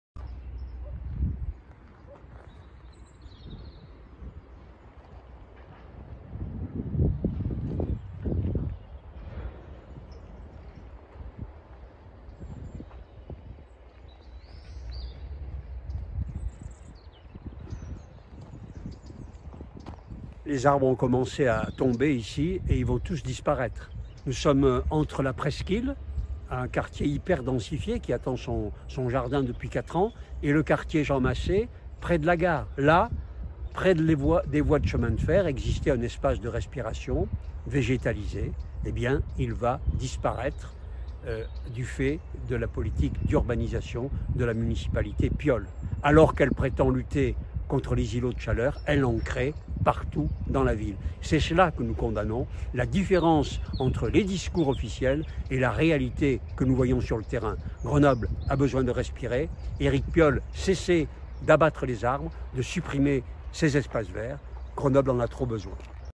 SAUVONS LES ESPACES DE RESPIRATION
Pendant que le Conseil Municipal de lundi va voir défiler nombre de délibérations pour se gargariser de " ville verte" d'espaces de respiration , de végétalisation, la municipalité poursuit la chasse aux espaces verts.
Grâce au PLUI ( plan d'urbanisme) qu'elle a mis en place partout ou cela est possible la densification se poursuit.
Ci-dessous entre le quartier Jean Macé et le quartier de la Presqu'ile, eux-mêmes très urbanisés , cet espace vierge, arboré est en train de disparaitre aussi.
Avec le réchauffement climatique notre cuvette Grenobloise va supporter des périodes de canicule de plus en plus longs. Nous devrions donc sanctuariser tous les espaces de respiration.
Quand Eric Piolle mettra -t-il ses actions en conformité avec ses déclarations?